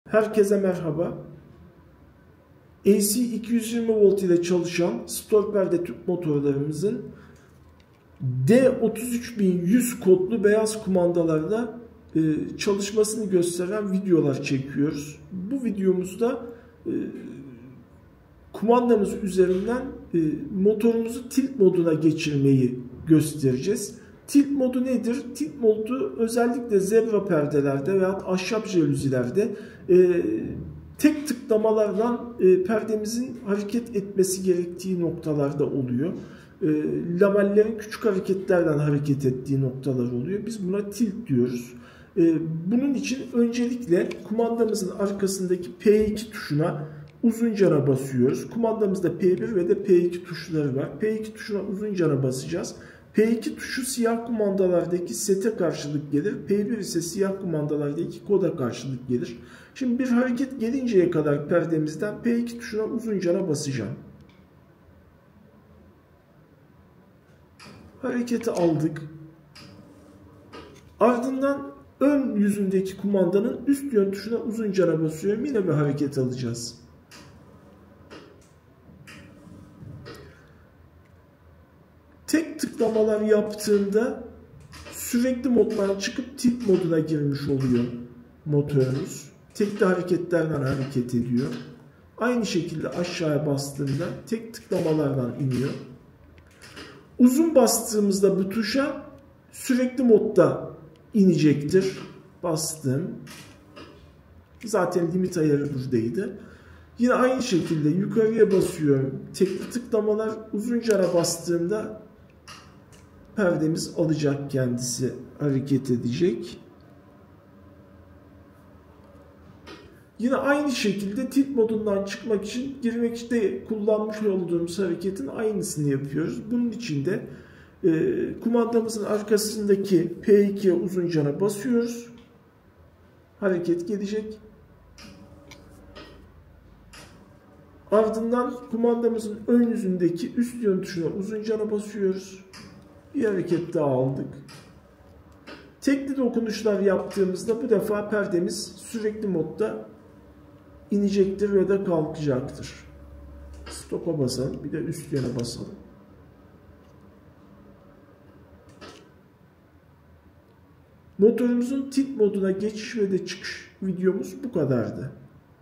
5 - D33100 KUMANDA İLE AC STOR PERDE TÜP MOTORLARININ TİLT MODUNA GİRİŞ VE ÇIKIŞI.
D33100 Kumanda ile AC Stor Perde Motorlarında Tilt Modu Ayarı | ACATAY PERDE MOTORLARI

Bu videomuzda, ACATAY PERDE MOTORLARI olarak NOVO marka AC stor perde motorlarınızda D33100 kodlu kumanda ile tilt moduna nasıl geçileceğini gösteriyoruz. Tilt modu, motorun her bir tıklamayla küçük hareketler yapmasını sağlar ve özellikle jaluziler ve zebra perdelerde slaytların tam kapanması veya ışığın hassas bir şekilde kontrol edilmesi için oldukça kullanışlıdır.

🎯 Tilt Modu Nedir ve Nasıl Geçilir? Motorlarımız, müşterilerimize ilk ulaştığında sürekli modda gelir. Bu modda, kumanda üzerindeki yön tuşlarına bir kez bastığınızda perde sürekli olarak hareket eder. Tilt moduna geçmek için:

Kumandanızın arkasındaki P2 tuşuna, motorunuzdan bir hareket alana kadar uzun süre basın.
Ardından kumandanızın ön yüzündeki yukarı yön tuşuna basın. Motorunuzdan bir hareket daha aldığınızda, motor tilt moduna geçmiş demektir.
Aynı işlemi tekrar yaparak motoru tilt modundan çıkarabilirsiniz.
Tilt modu, özellikle jaluziler ve zebra perdelerde hassas kontrol sağlar ve ışığı dilediğiniz gibi ayarlamanıza olanak tanır. Daha fazla bilgi ve ürünlerimiz için web sitemizi ziyaret edin